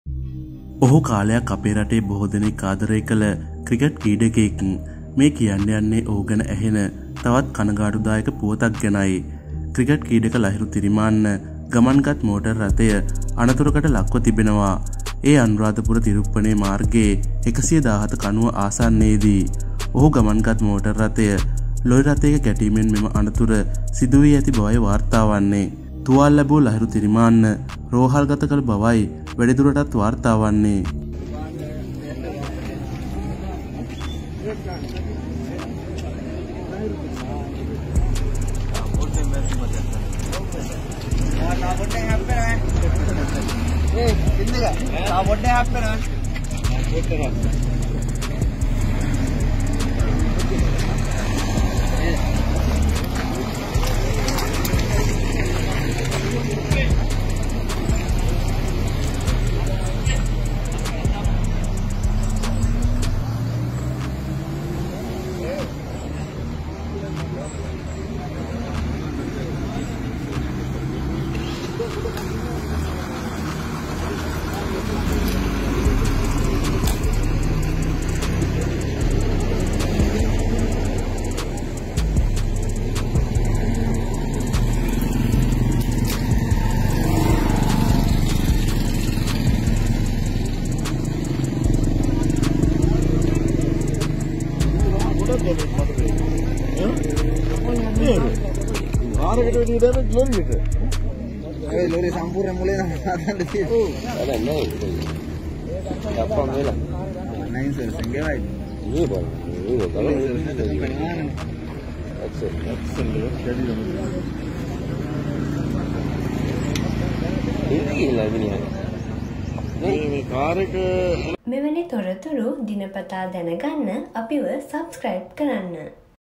ළහිරු තිරිමාන්න අනුරාධපුරේදී දරුණු රිය අනතුරක | lahiru thirimanna
The aim of our channel is to provide our viewers with the latest information in the world of art

lahiru thirimanna news